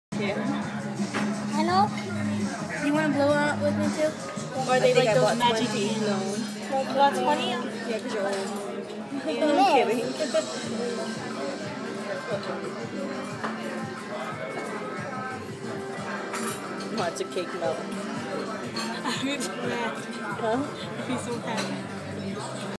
Aiden @ Longhorns (3/15/12)
While waiting for food to be served... Keeps the lil guy busy lol